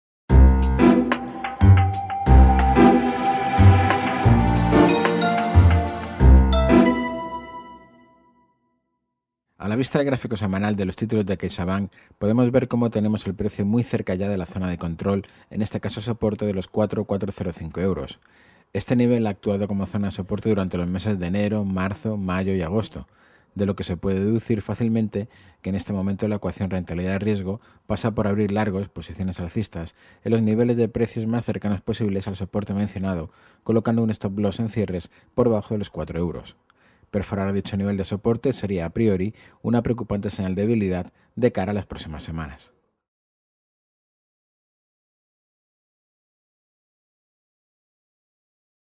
Vídeo análisis técnico Caixabank
Las caídas de las últimas sesiones están acercando con peligro a los títulos de CaixaBank a la zona de soporte de los 4-4,05 euros.

Perforar este nivel, al cierre y con holgura, sería un jarro de agua fría para el título.